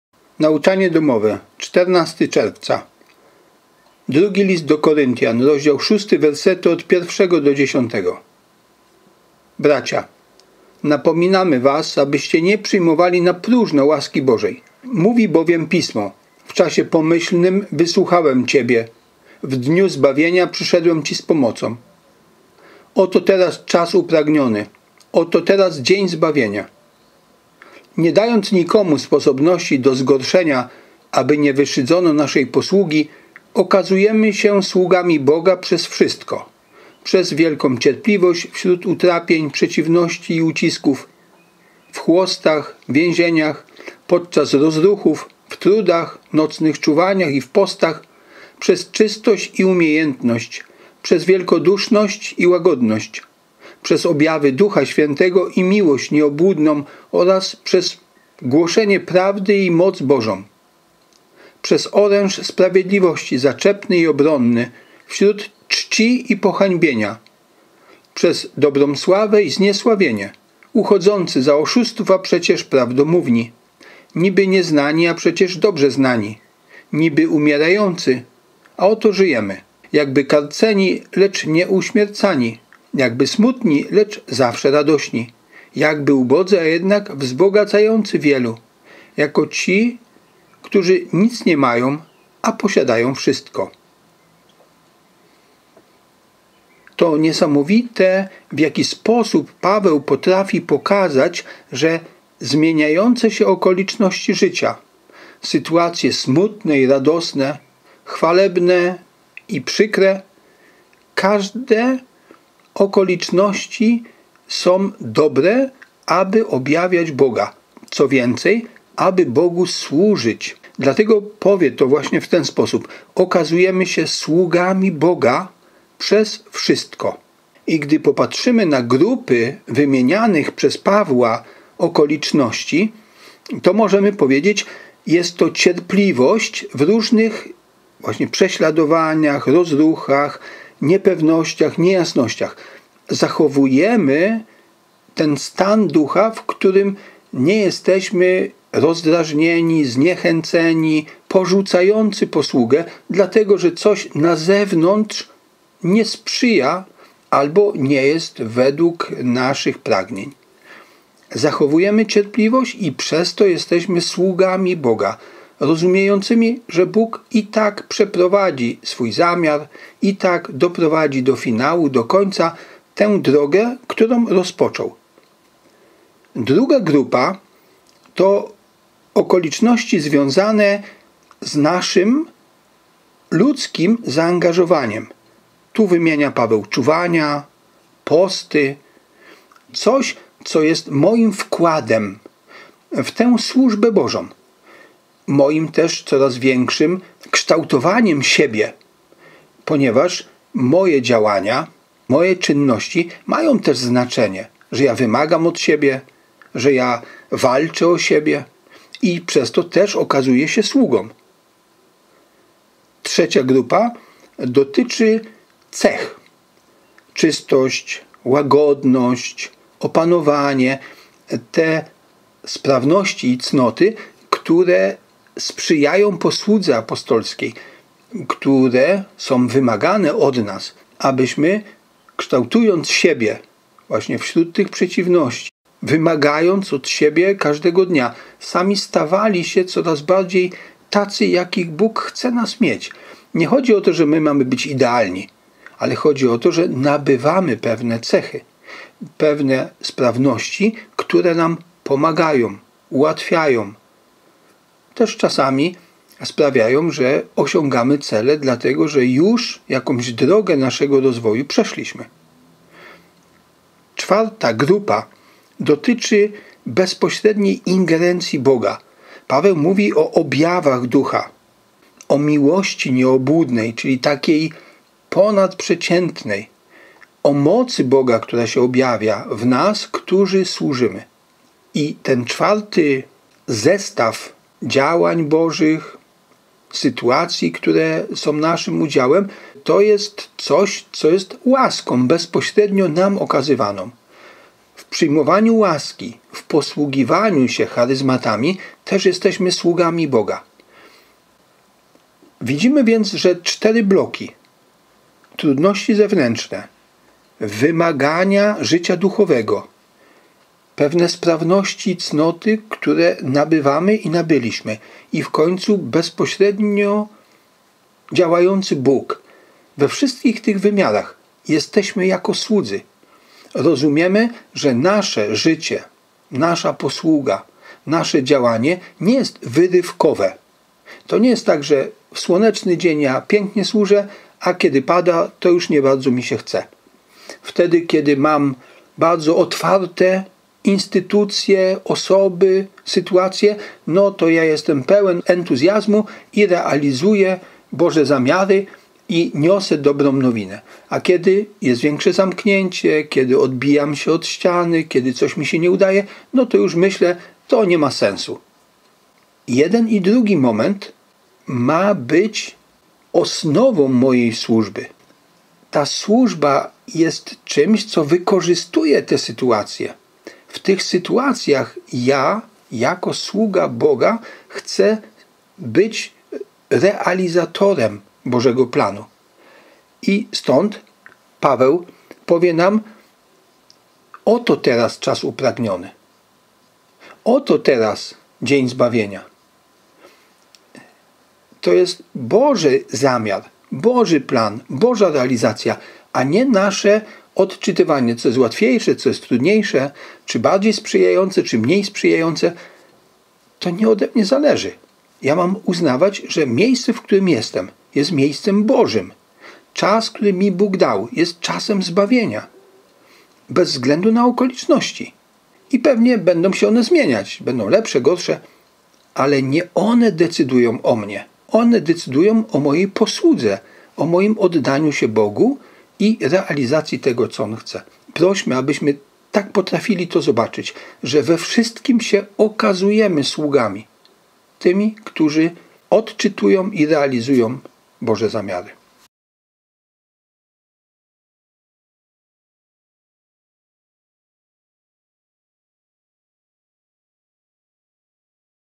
NauczaniaDomowe – L1#72 - Słudzy Boga - ArturSepioło – 14.06.2021
Spotykają nas w życiu zarówno prześladowania, jak i pochwały. We wszystkim i w każdych okolicznościach mamy okazywać się sługami Boga.

Informacje ogólne:
Prowadzący:
Kamera:
Grafika kanału: Robert Mierzwiński